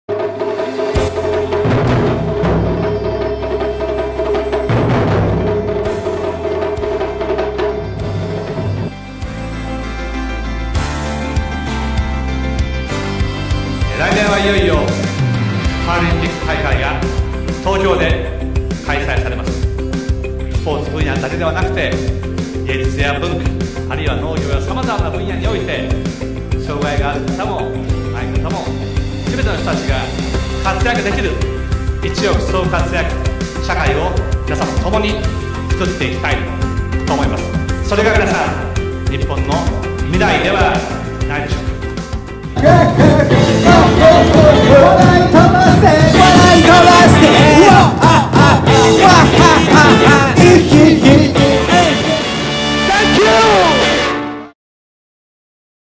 安倍総理と障害者の集いー令和元年5月21日
安倍総理は、「安倍総理と障害者との集い」に出席しました。その様子を動画にまとめましたので、ぜひご覧ください。